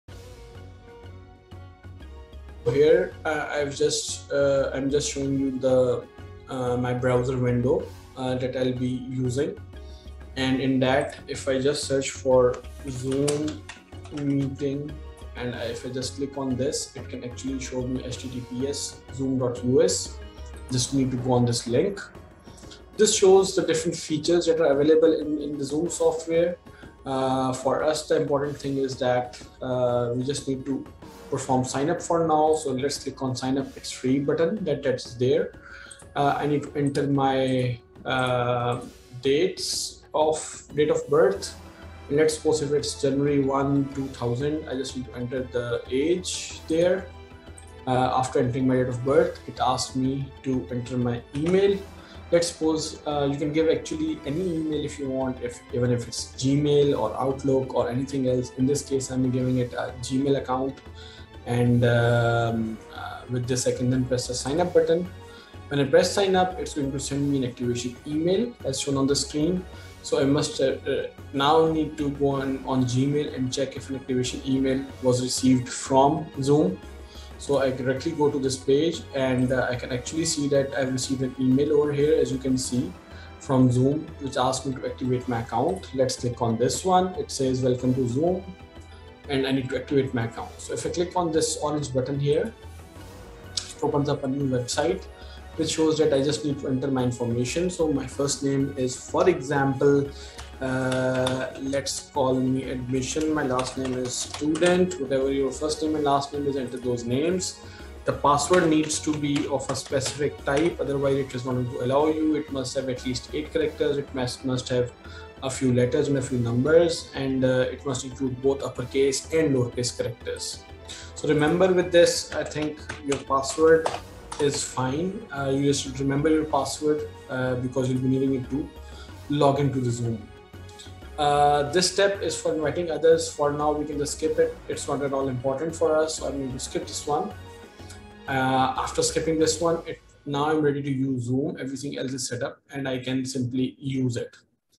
Undergrad GIK Admissions Video 1 English - Setting Up Zoom Online Account for the test
This video is especially meant for GIK Undergraduate Admission test - The video shows how you need to set up your account in zoom in English language.